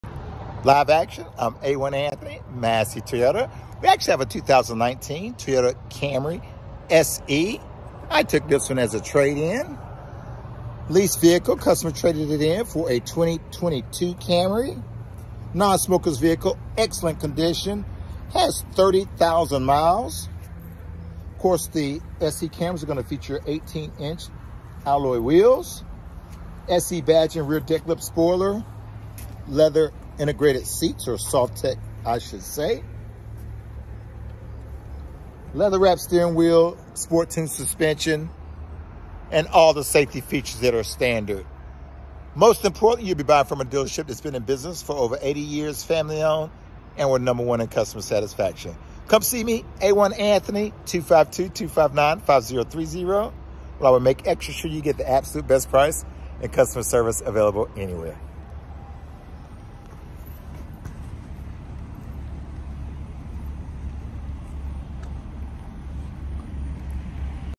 A-1 Anthony of Massey Toyota demonstrates a 2019 Toyota Camry SE
A-1 Anthony Patrick of Massey Toyota demonstrates a 2019 Toyota Camry SE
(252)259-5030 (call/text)